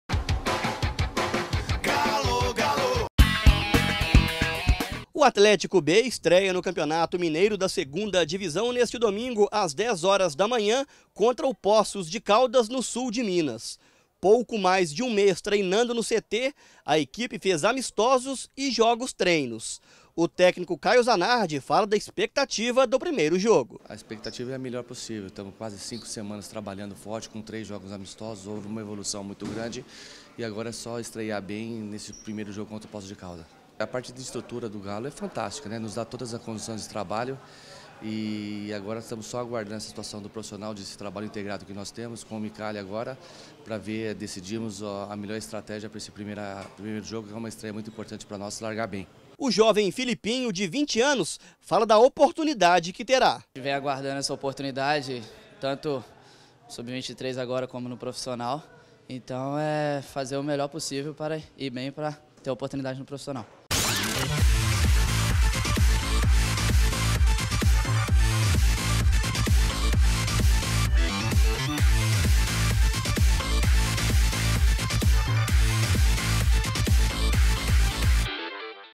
28/07/2017 Atlético B encerra preparação para estreia no Campeonato Mineiro
Atlético B encerra preparação para estreia no Campeonato Mineiro da segunda divisão